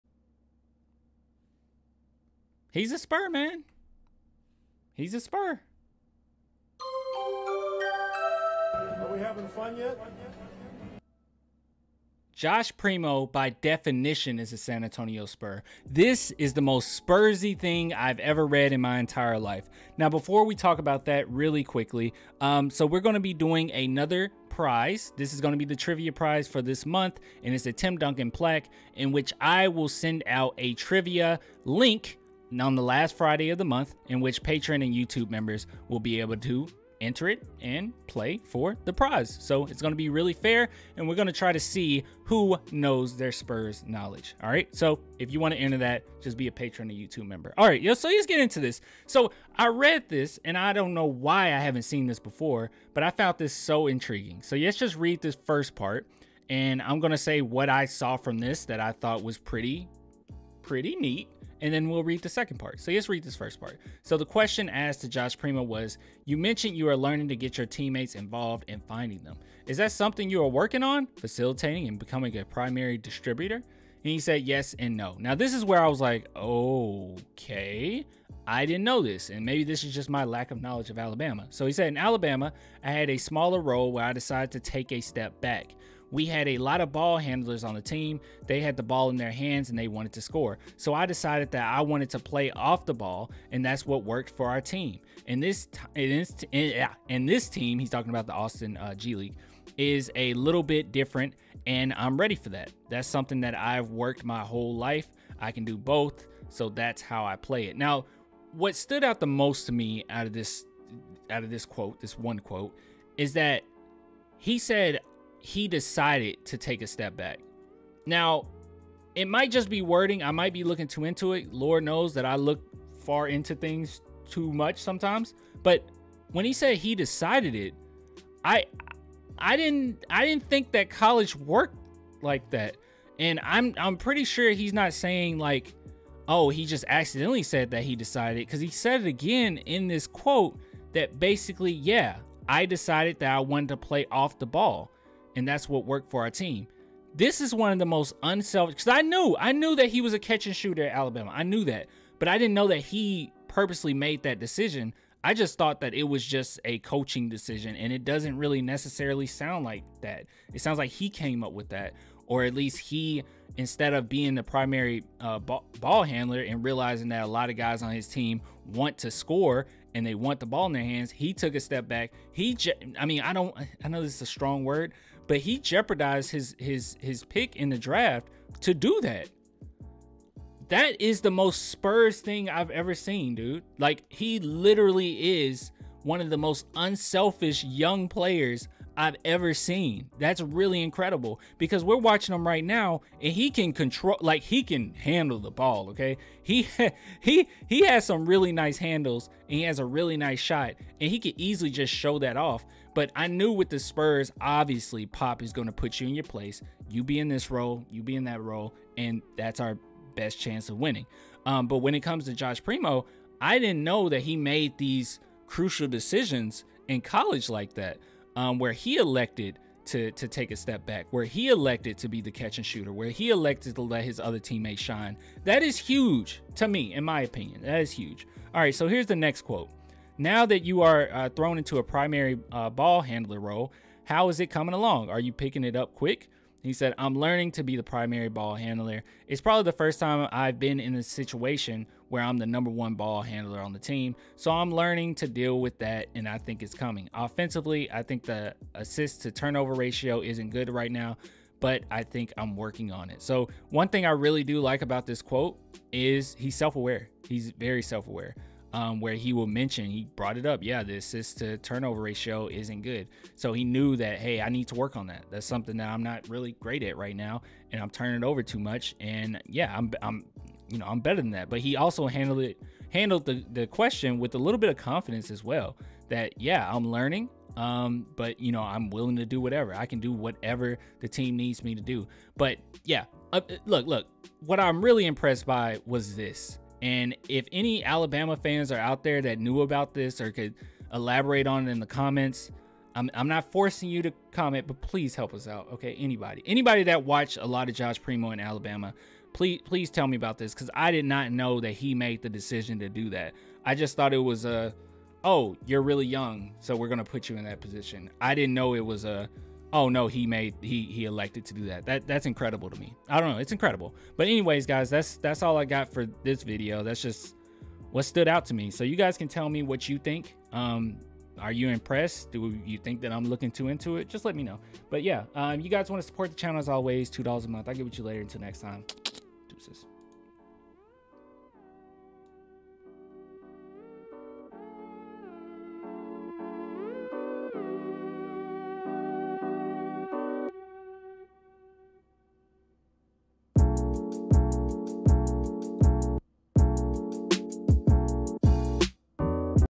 Josh Primo Is UNSELFISH and SELF-AWARE!
Joshua Primo is by definition a San Antonio Spur! GO SPURS GO!

//About me:
0:00 Intro
0:21 Trivia Prize
0:48 Primo Is So Humble
4:44 Outro